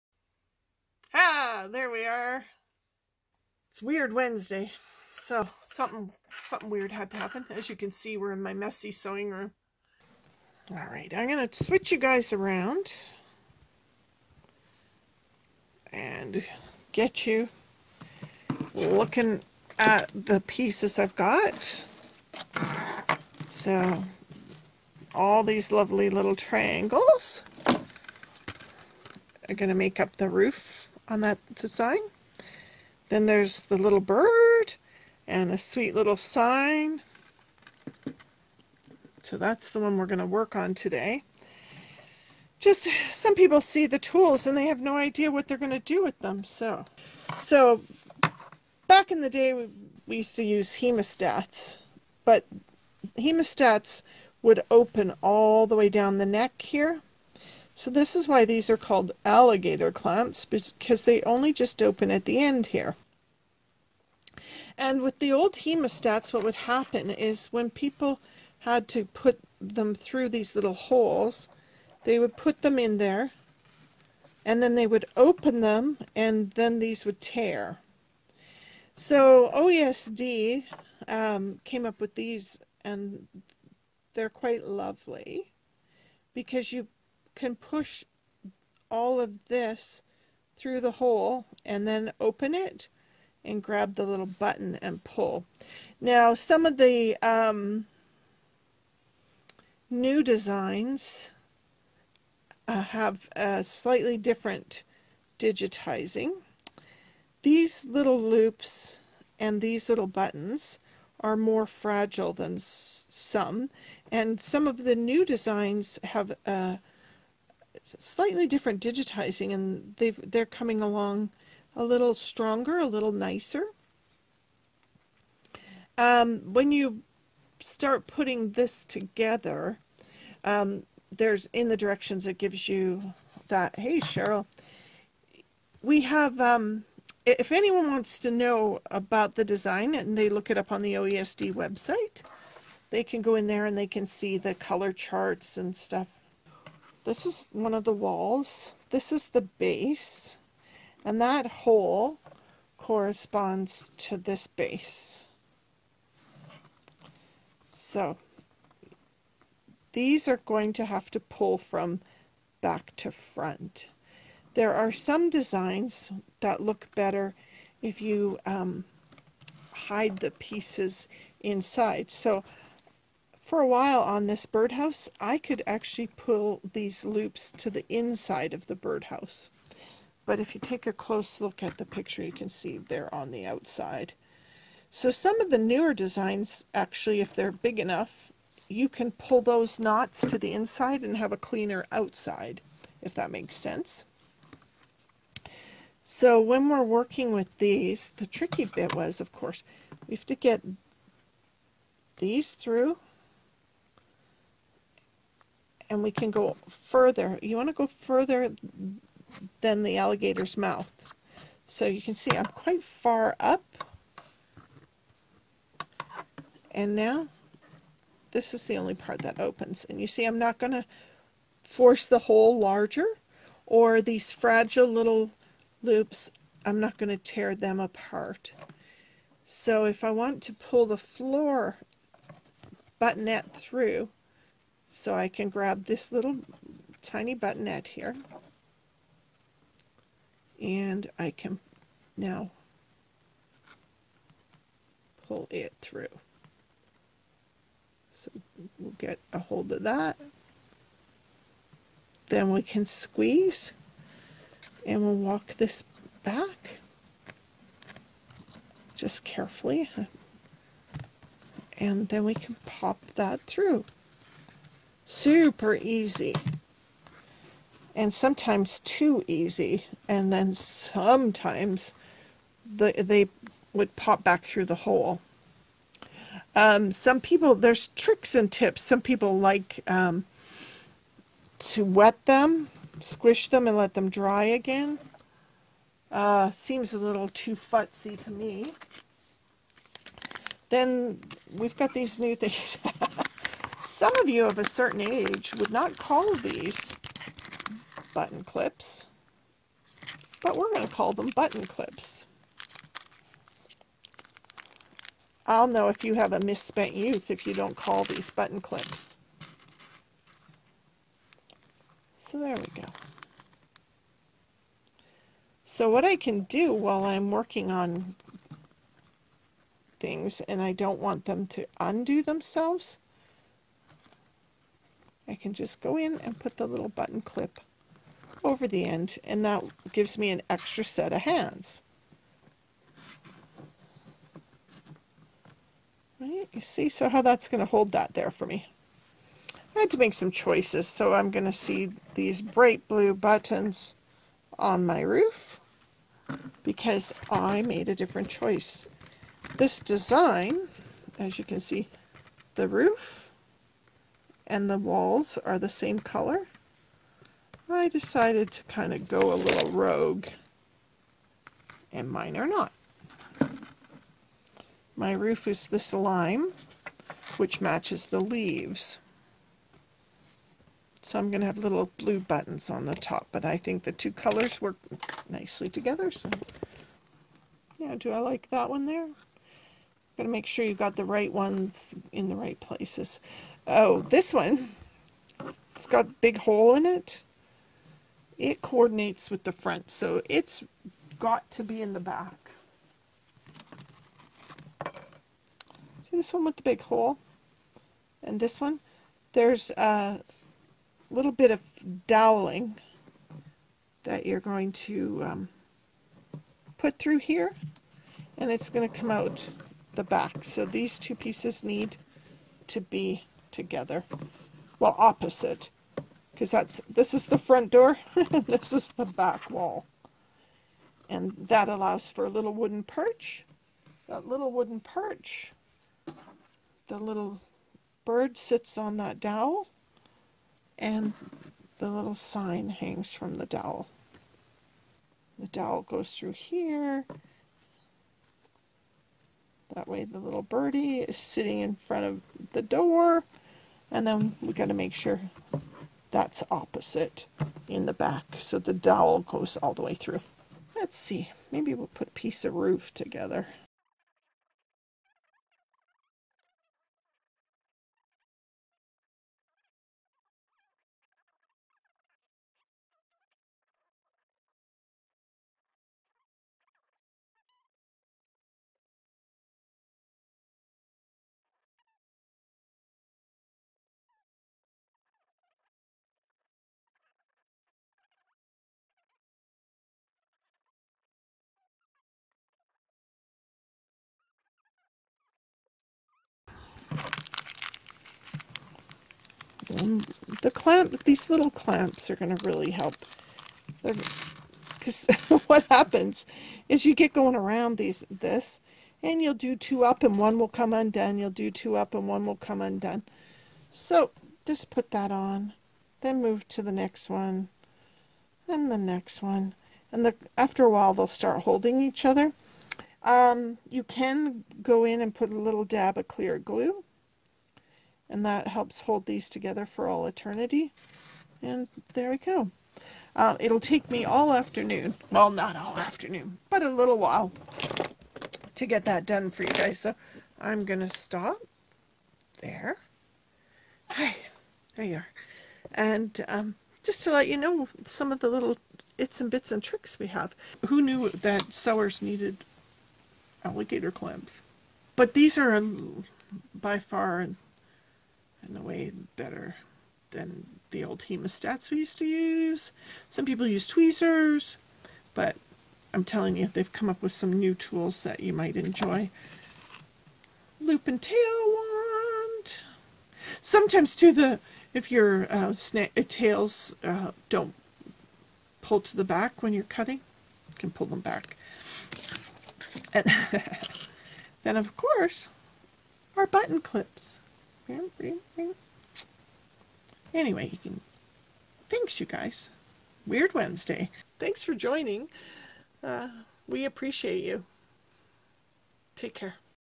Weird Wednesday: Freestanding Lace Birdhouse
Anne gives a quick demo on how to put together a freestanding lace birdhouse, as well as some tips on working with lace ornaments